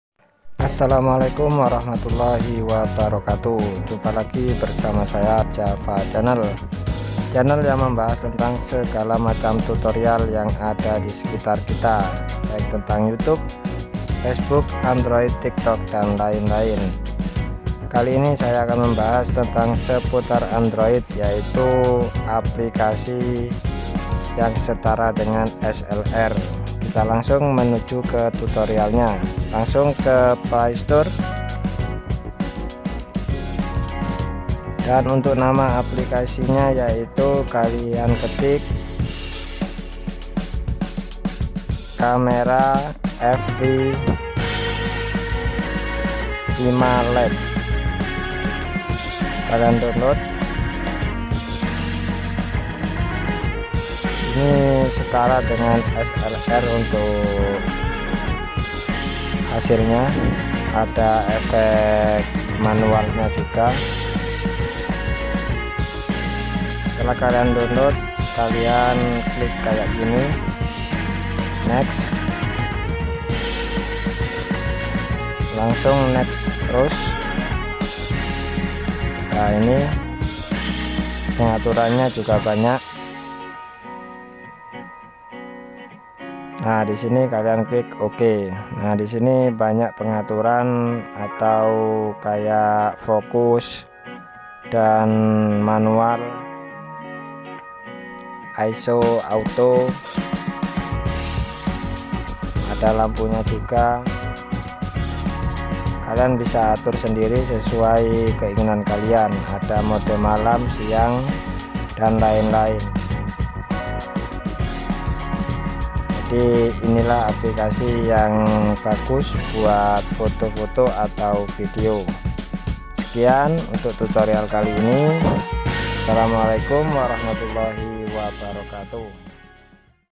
APLIKASI KAMERA HP SEPERTI DSLR
Berikut video tutorial cara mendapatkan aplikasi kamera hp seperti dslr

Alat perekam        : hp Oppo A7
Aplikasi edit          :Kine master
Edit THUMBNAIL :pixel lab
Edit foto                 : SNAPSHEED
Aplikasi kamera    :open CAMERA